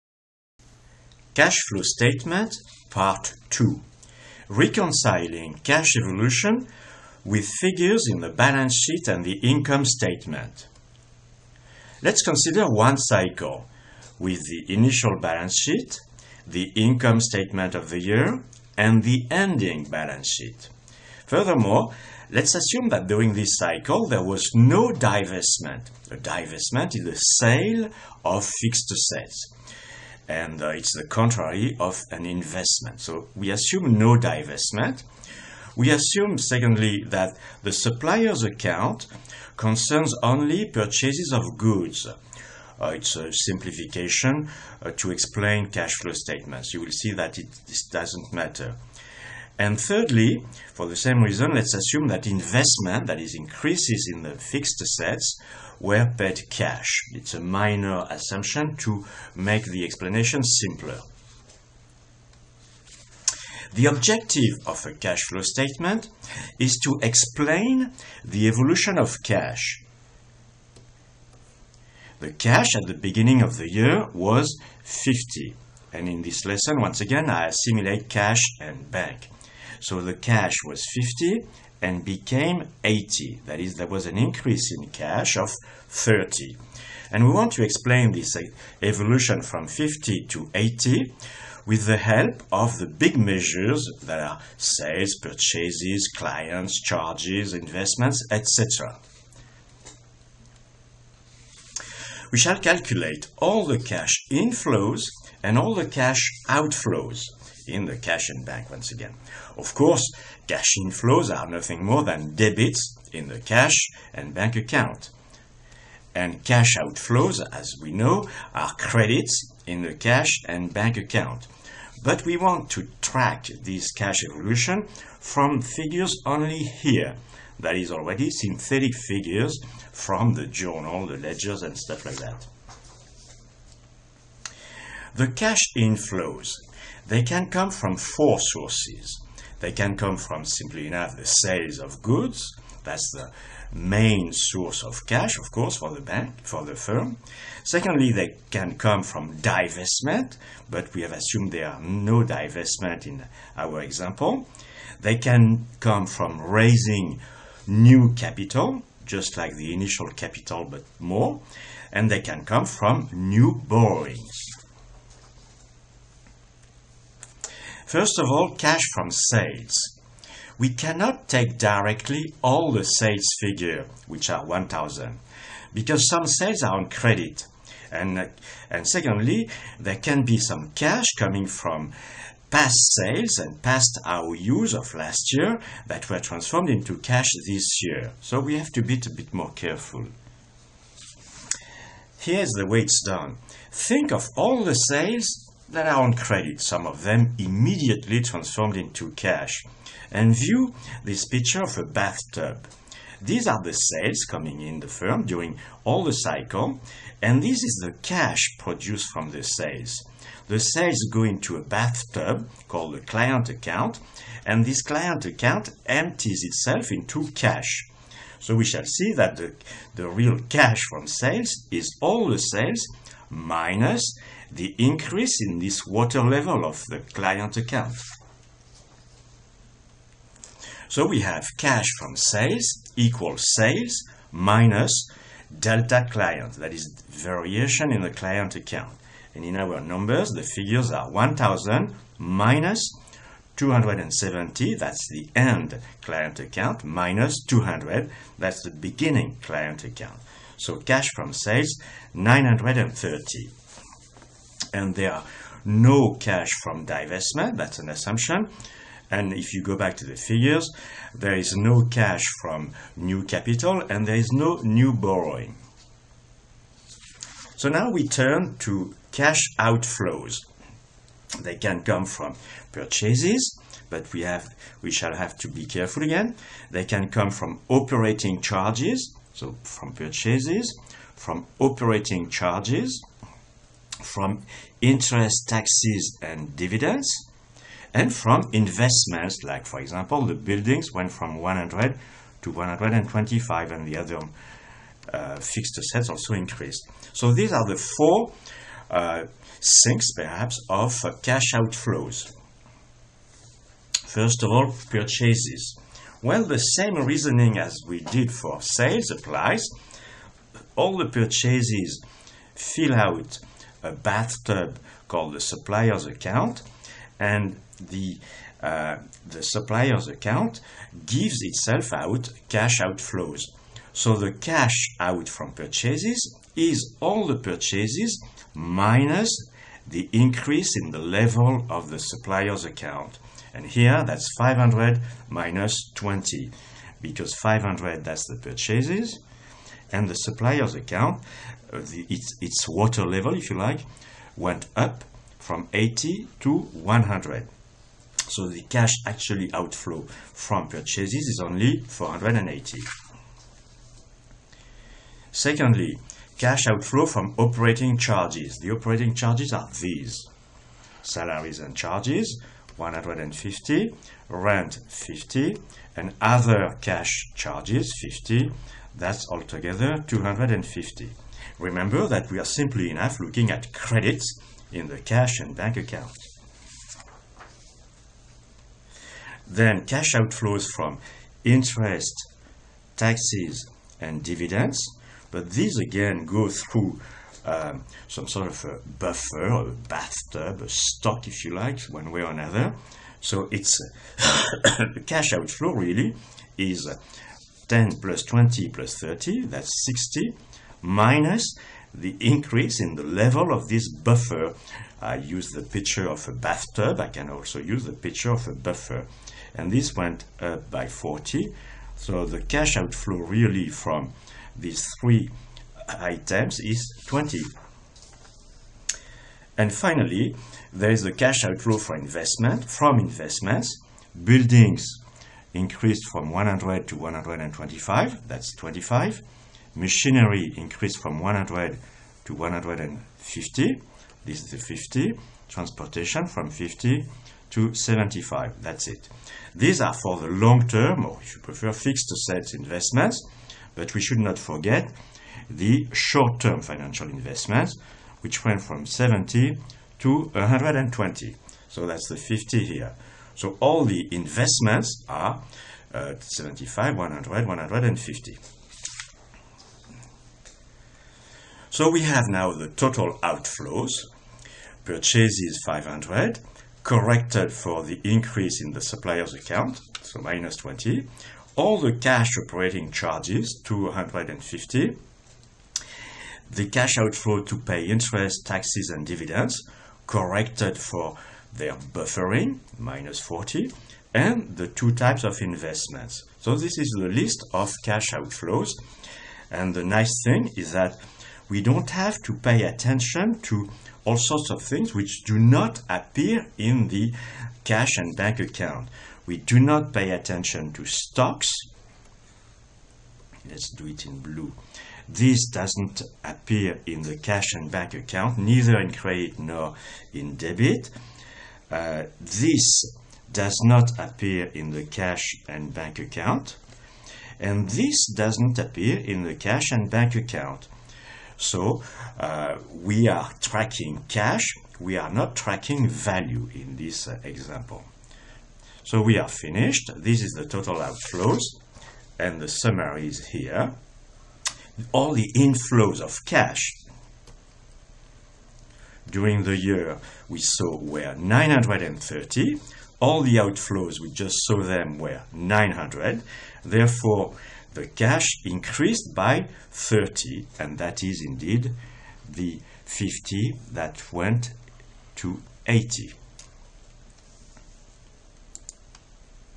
cash flow statement
we explain the evolution of cash over an accounting cycle with the big figures in the Balance Sheet and Income Statement (sales, purchases, clients, charges, etc.) without the need of the complete ledger. We explain how the clients account and the suppliers account are "buffering" stocks for cash movements.